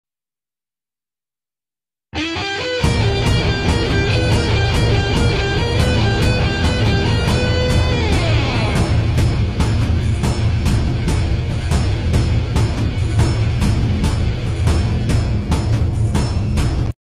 TikTok: WonderWoman
Hell yeah!! a sexy WW, still trying to learn how to draw bodies.

Art by Medel.
Software: ClipStudio Paint Ex